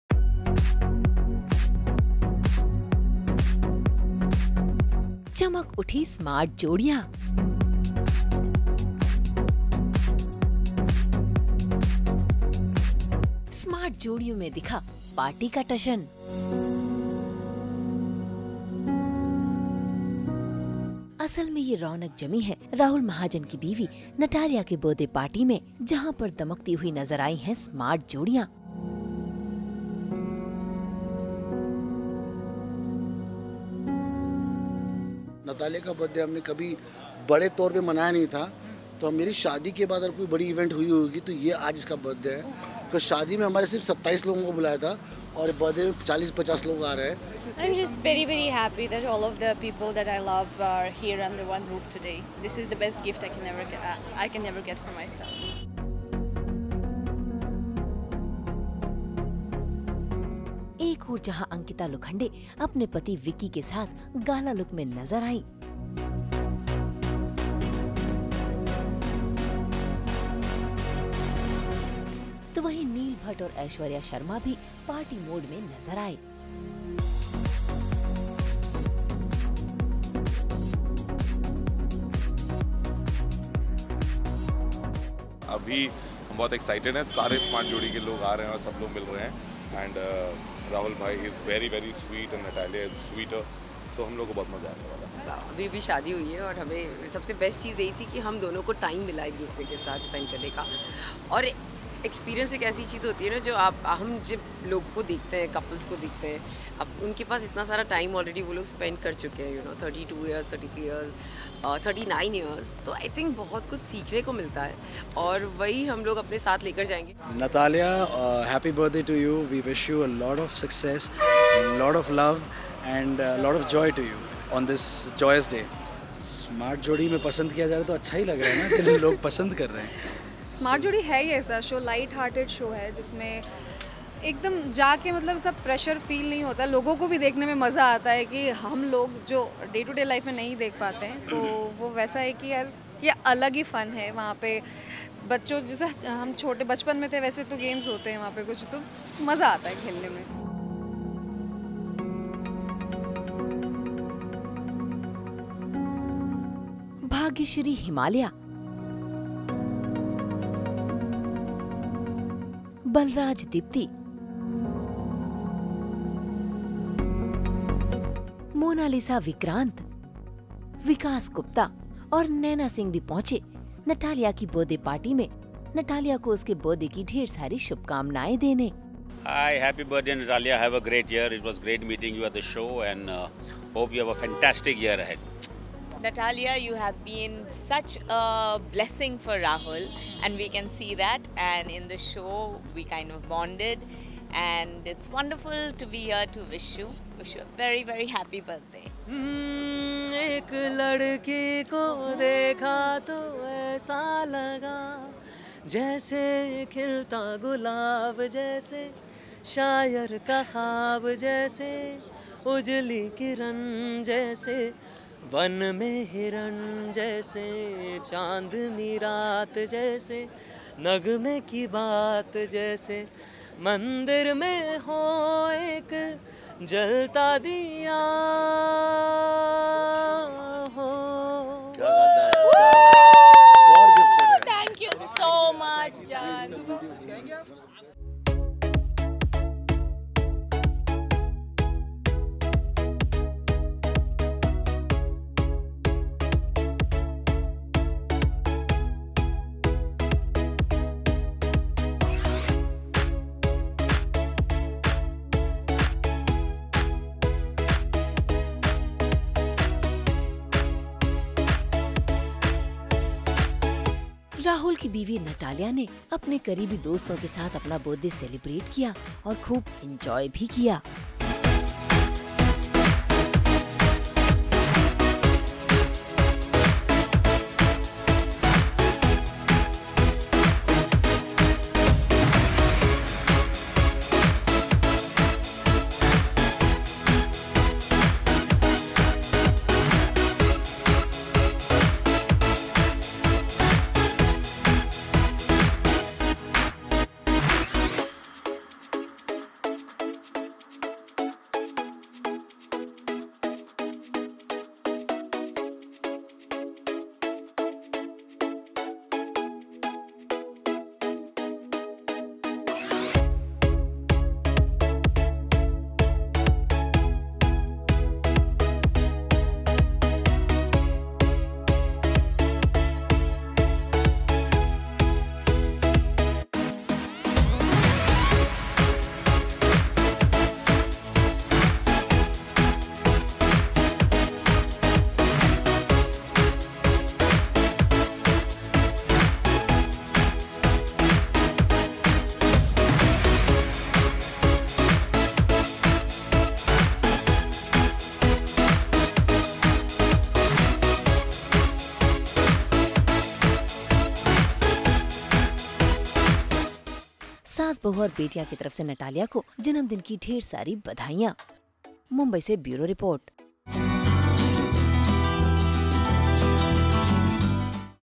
Rahul Mahajan Hosts A Grand Birthday Party For Wife Natalya.
check Out Rahul Mahajan Hosts A Grand birthday Party For Wife, Natalya, Many TV Celebs Attend The Bash.